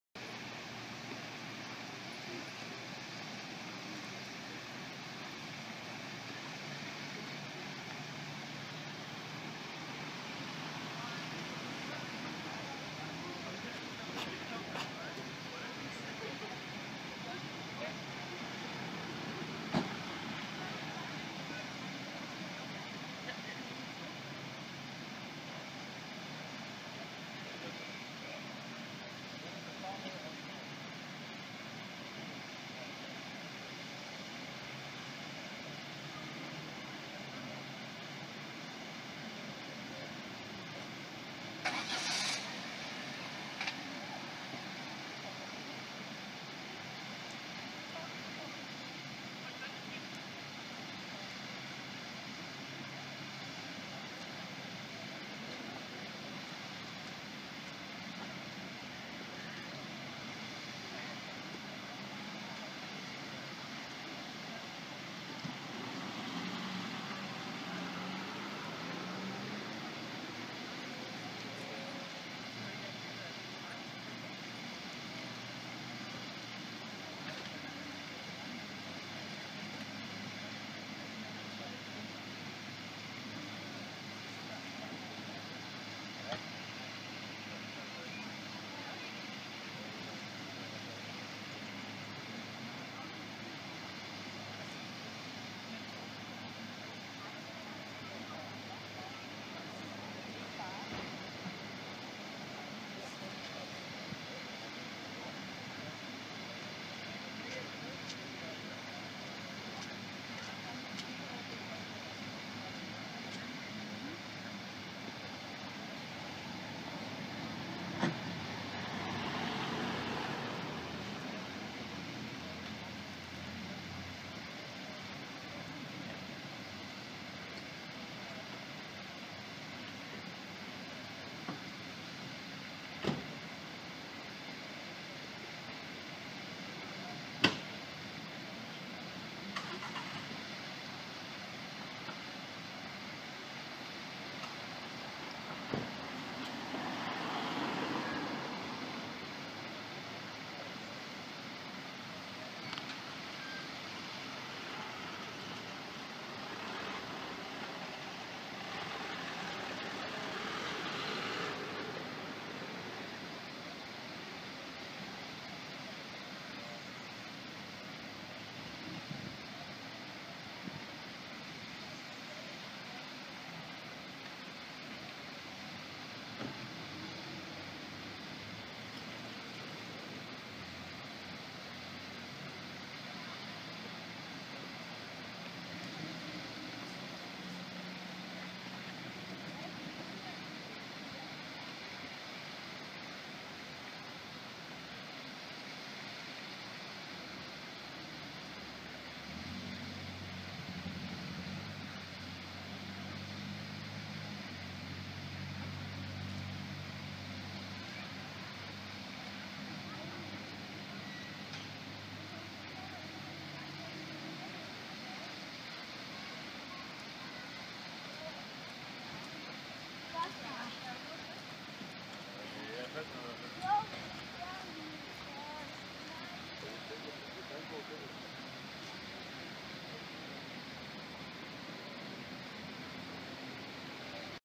Churchillparken with Gefionspringvandet and Den Engelske Kirke June 15th 2014
A movie of Churchillparken (The Churchill Park) in Copenhagen that also contains Gefionspringvandet (The Gefion Fountain) and Den Engelske Kirke (St. Alban's Church).
The movie is recorded at June 15th 2014.